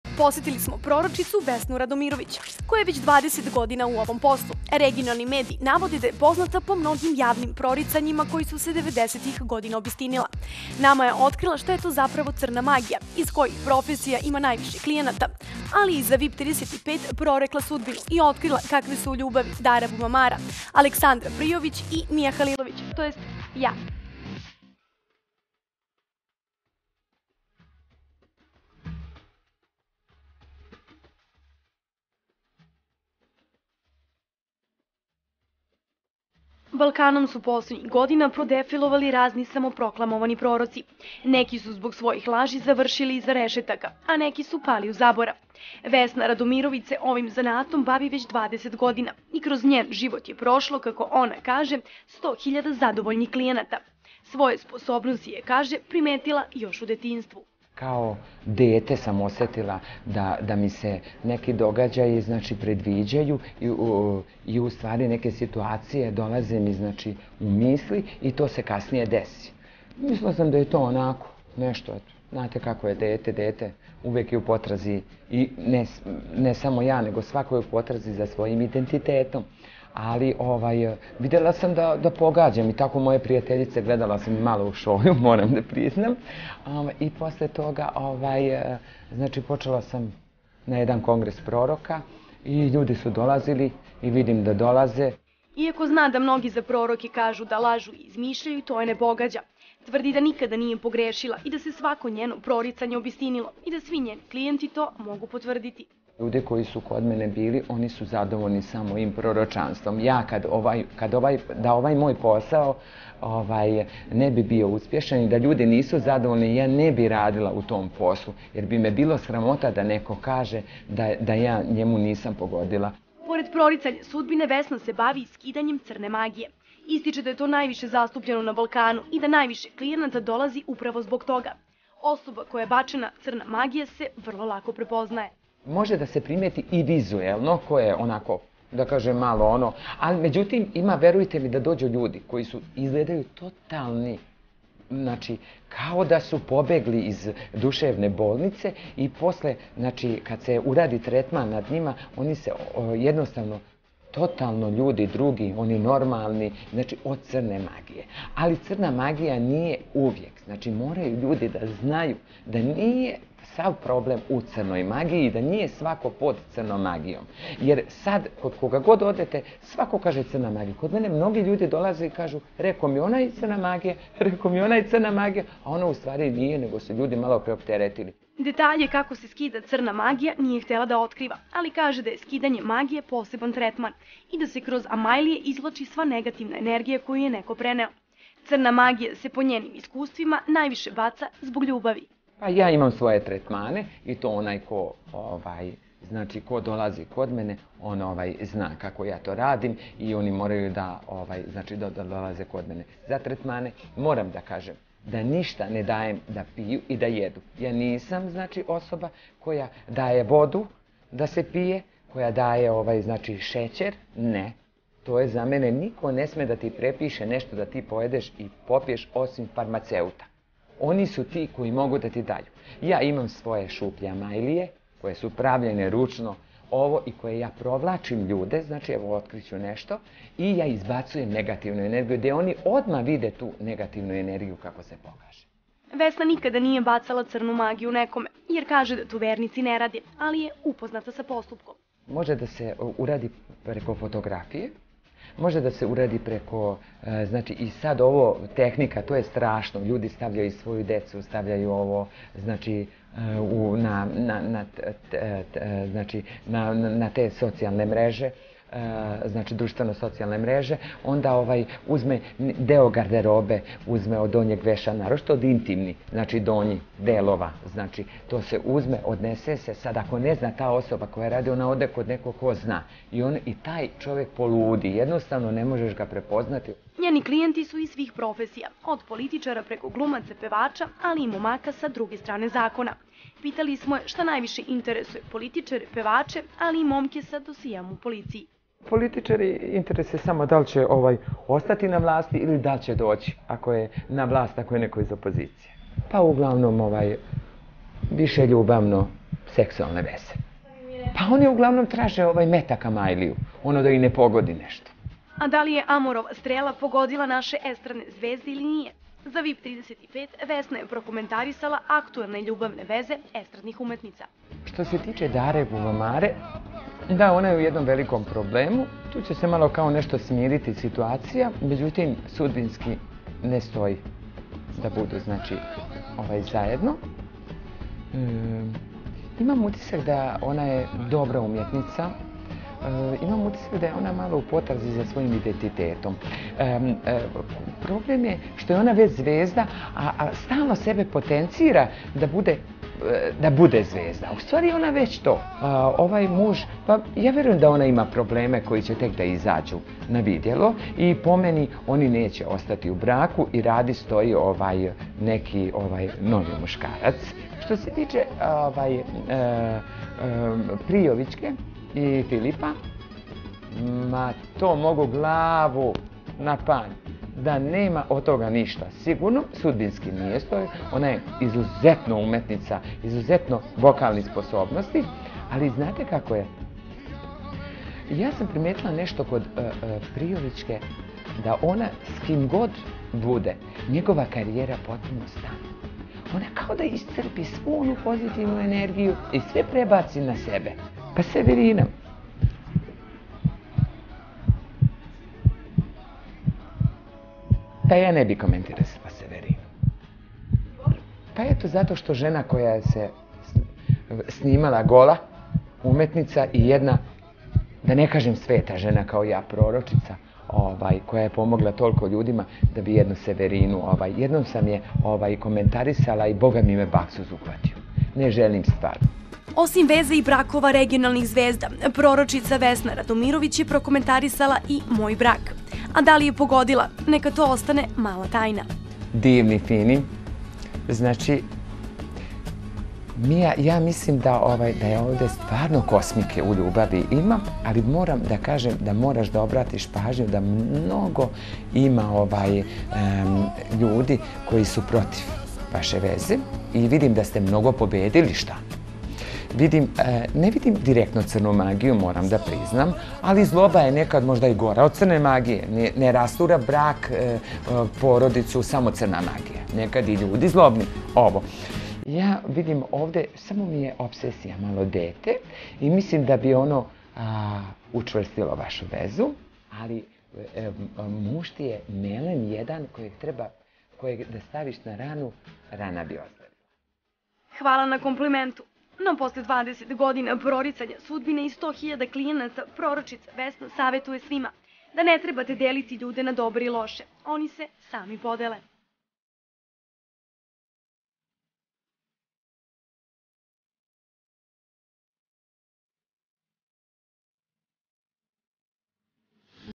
Proročica Vesna Radomirović otkriva šta je crna magija i kakve su estradne pjevačice u ljubavi
Posjetili smo proročicu Vesnu Radomirović koja je već 20 godina u ovom poslu. Regionalni mediji navode da je poznata po mnogim javnim proricanjima koja su se 90-ih godina obistinila. Nama je otkrila šta je to zapravo crna magija, iz kojih profesija ima najviše klijenta ali je i za VIP35  prorekla sudbinu i otkrila kakve su u ljubavi Dara  Bubamara, Aleksandra  Prijović i Mia Halilović, tj. ja.